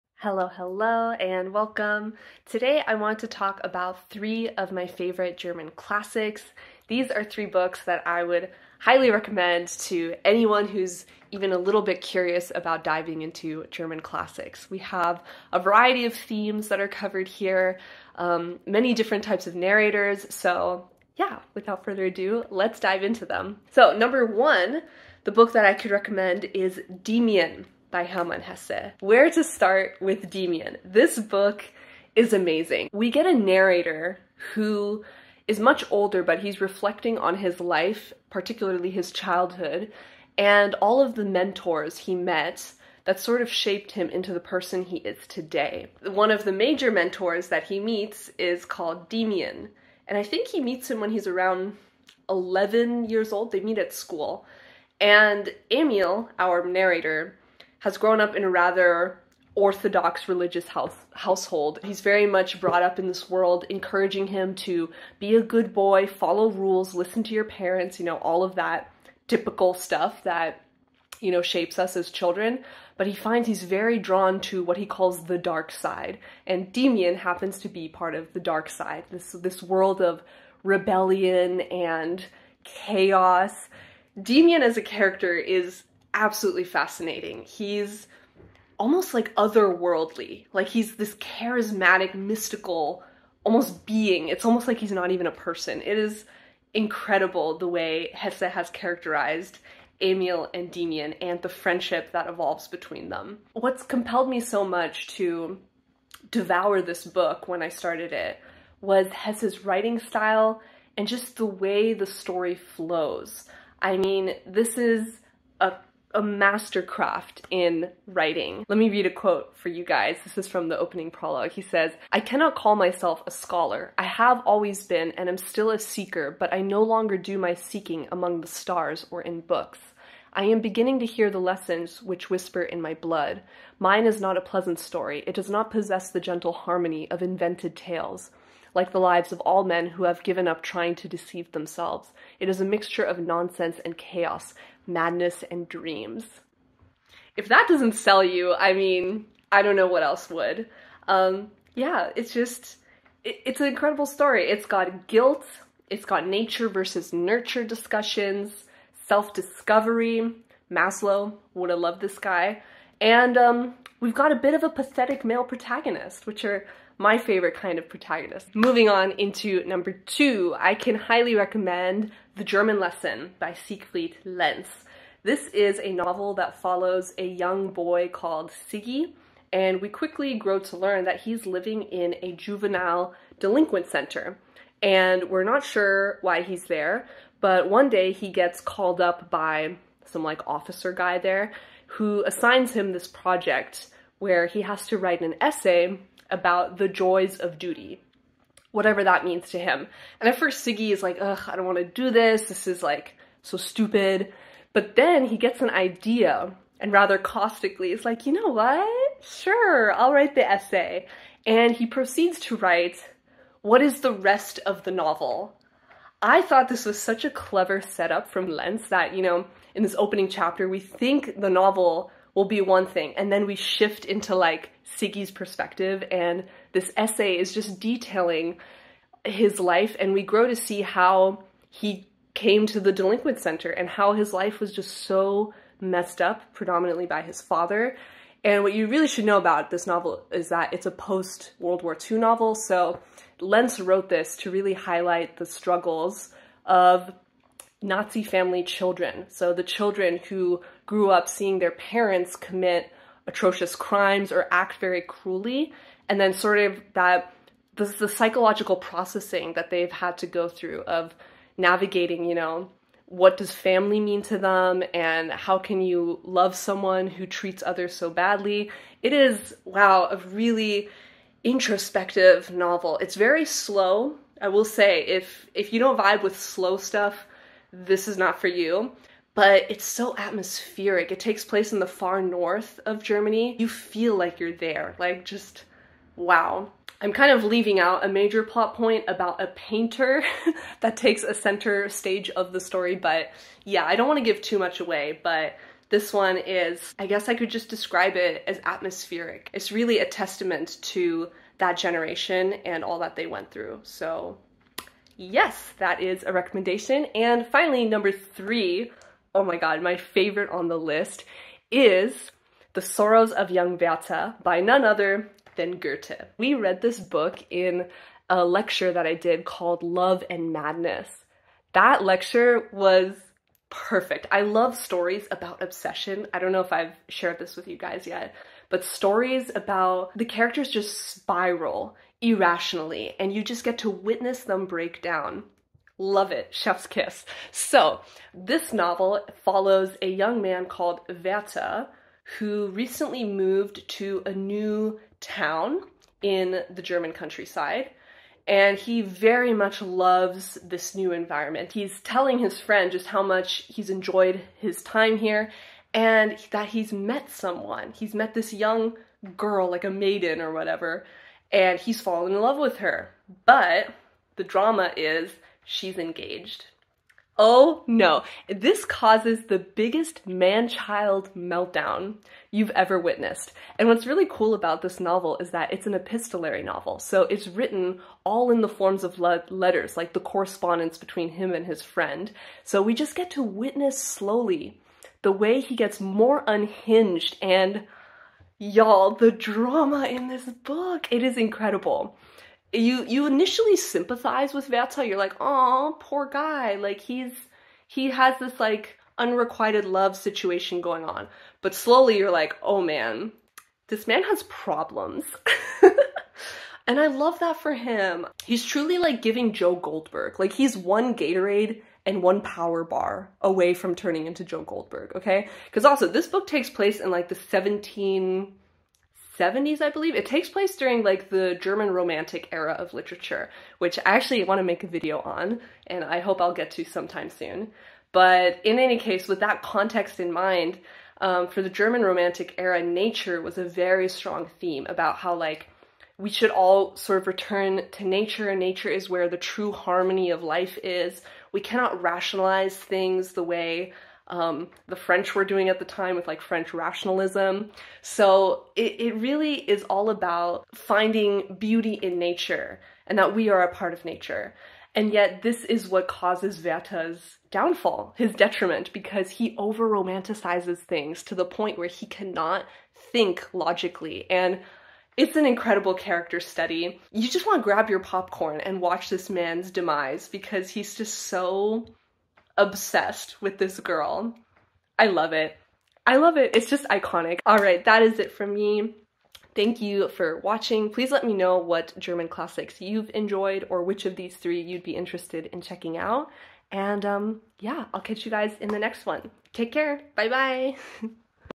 my top 3 favorite german classics (books on obsession, existentialism, and more!!)
I had fun rambling about these. hope you did too!!!

timestamps:
0:00 - intro
0:23 - demian
2:48 - the german lesson
5:07 - the sorrows of young werther